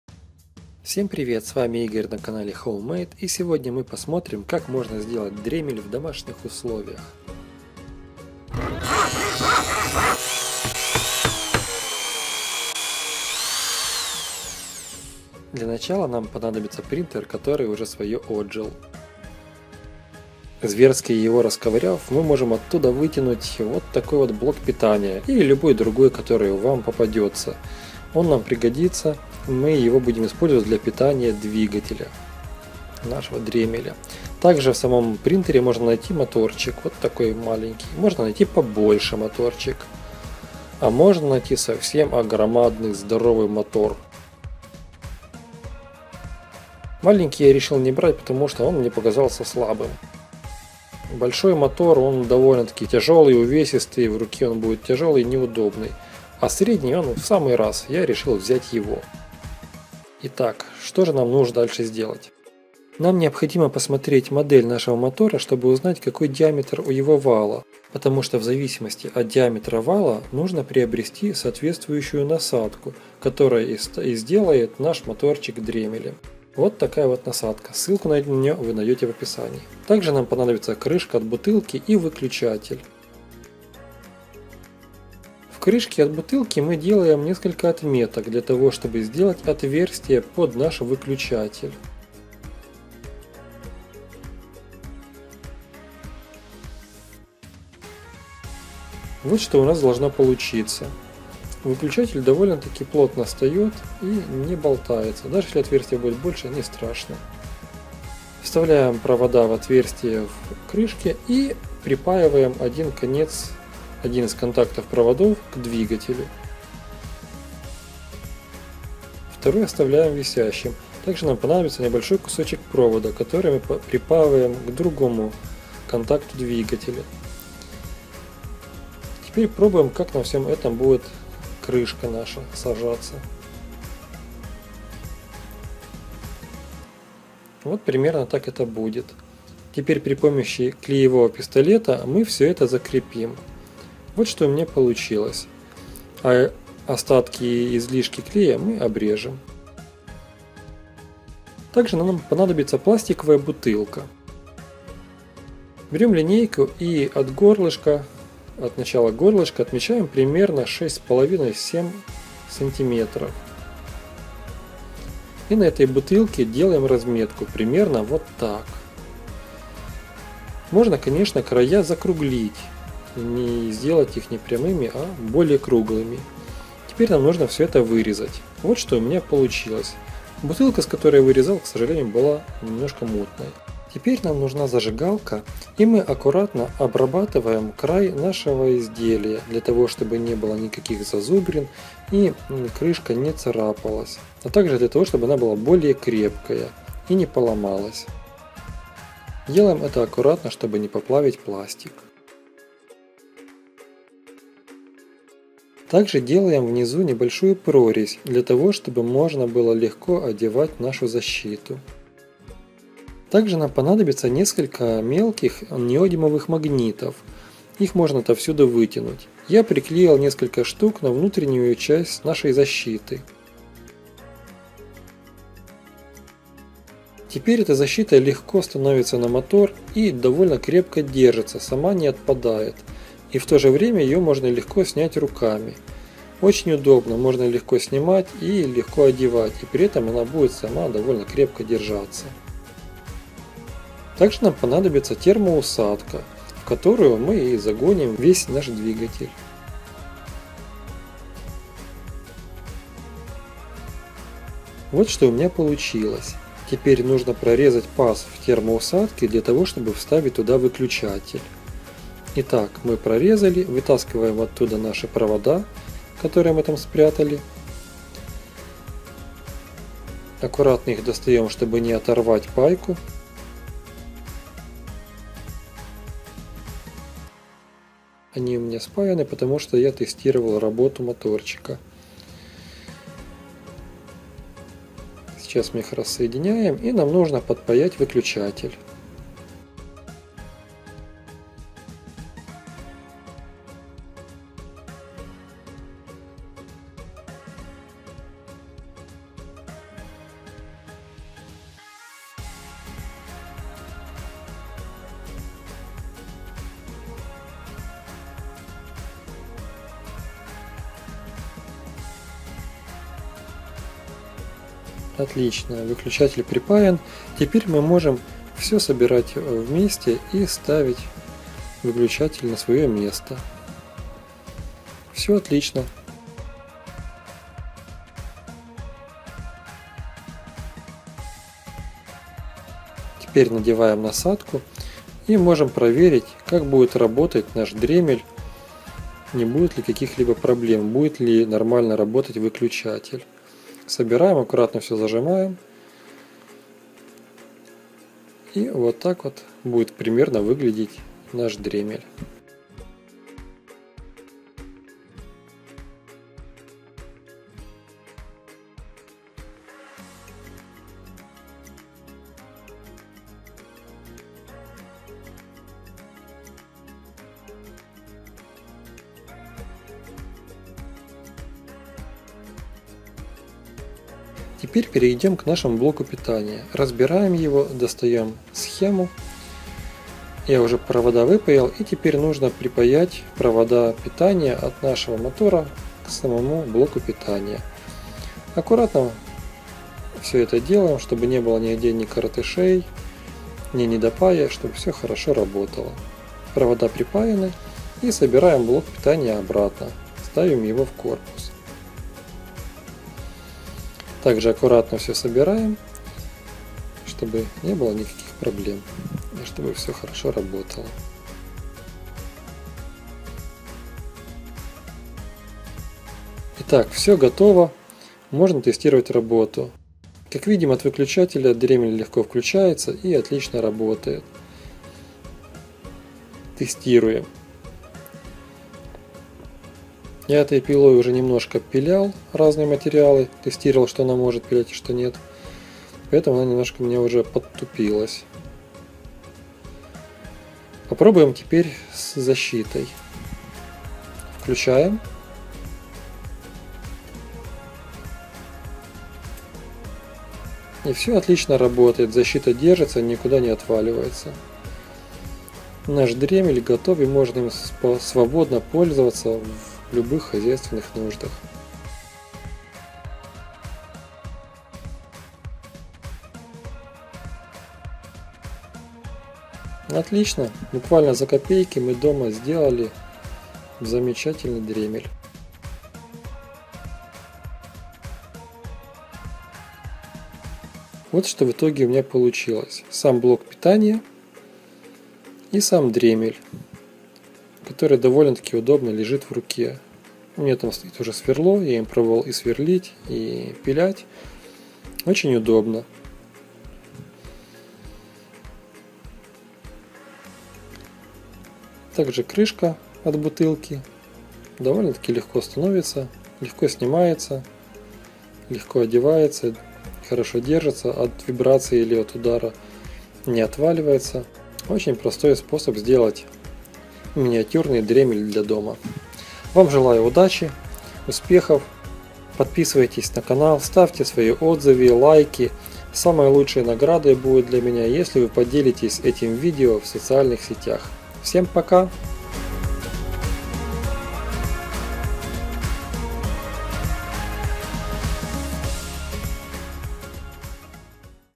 Мини дрель / дремель своими руками из старого принтера!
Если у вас дома завалялся старый принтер, то из него легко можно сделать мини-дрель (дремель).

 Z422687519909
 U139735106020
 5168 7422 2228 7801 Приват Банк (Украина)